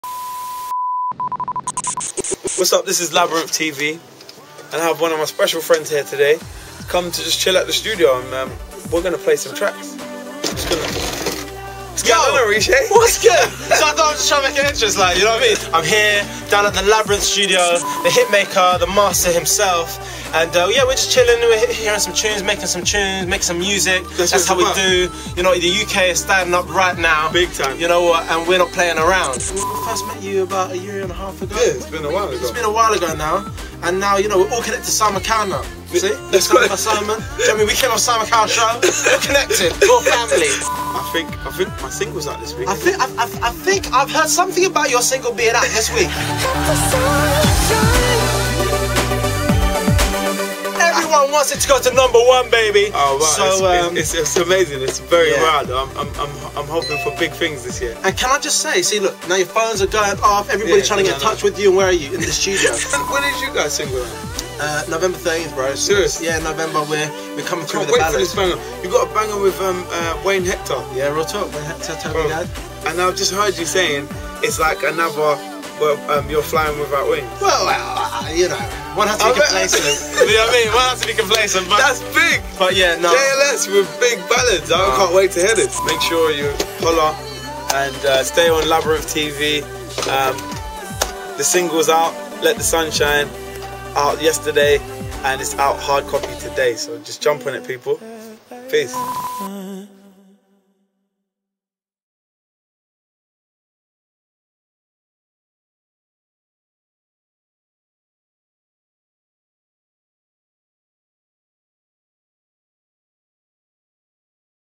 Labrinth: VLOG #4
Taking time out from working hard in the studio with Oritsé Williams (JLS).